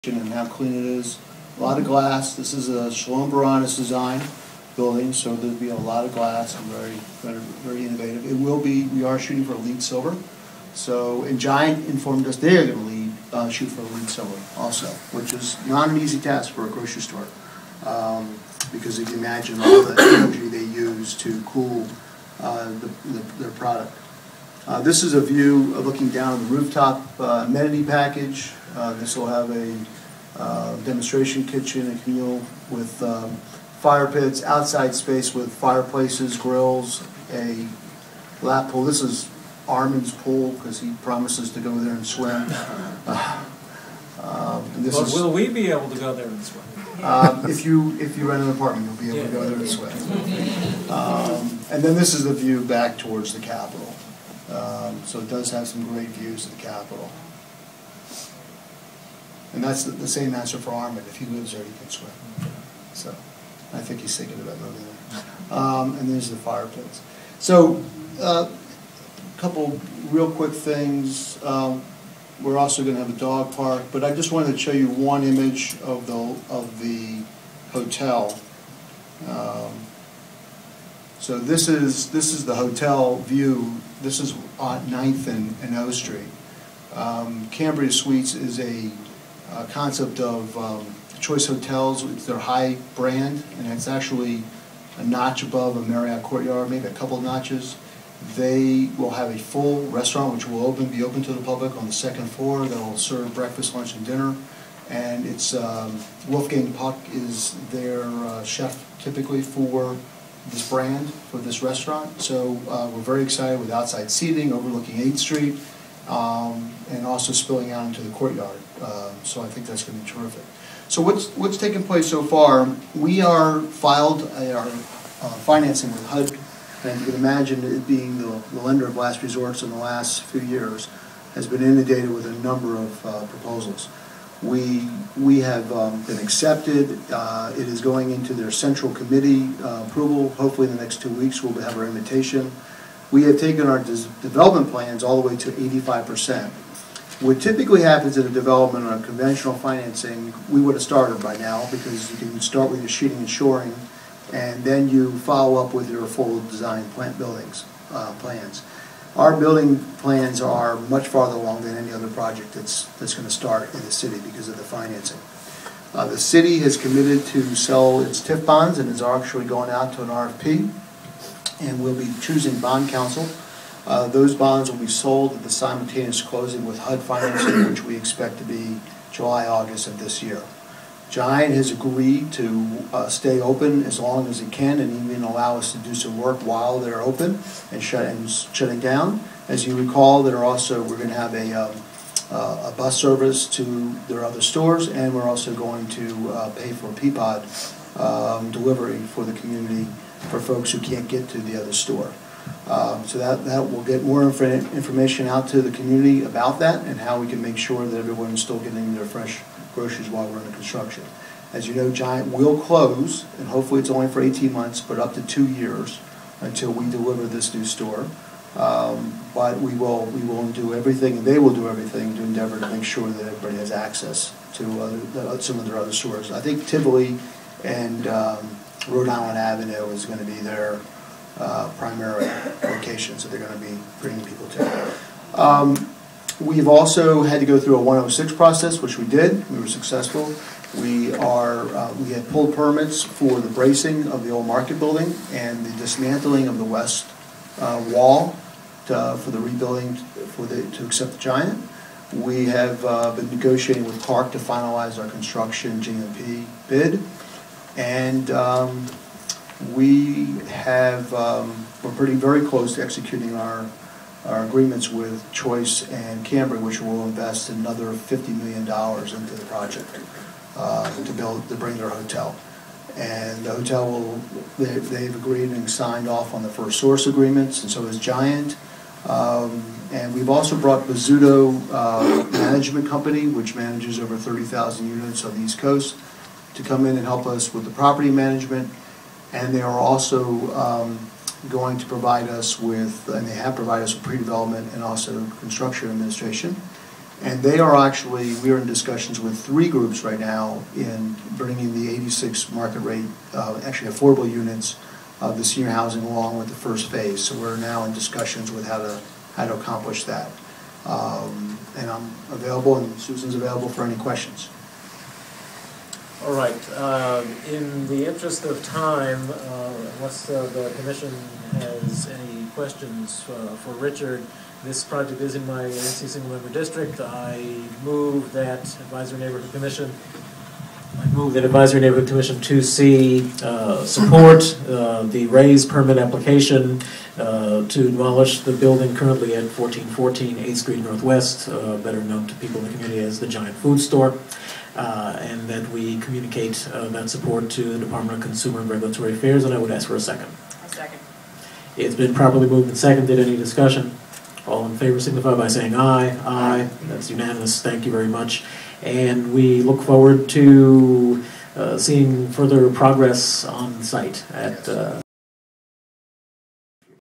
City Market At O, ANC2C March 2, 2011, Part 2
Roadside Development gave a descriptive tour of the latest plans for CityMarket at O at this month's Advisory Neighborhood 2C meeting and were give a raze permit to demolish to current Giant grocery store which will be replaced by a new Giant grocery which is expected to be the largest in the District when completed. New plans feature rooftop fire places, pool, dog park, spectacular unobstructed views of Capitol and city.